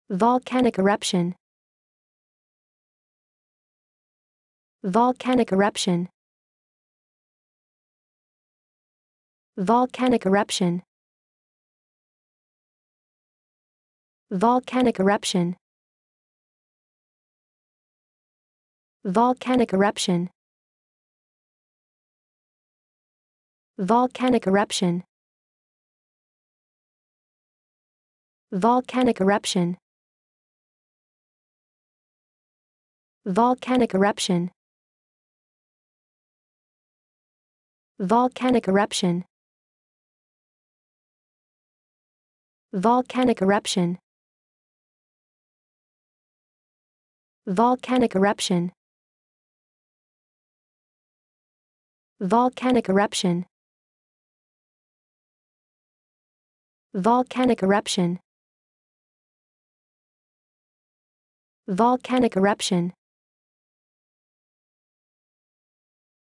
Como dizer 'erupções vulcânicas' em ingles?
Como dizer erupções vulcânicas em Ingles? Como se pronuncia volcanic eruption? How to pronounce volcanic eruption?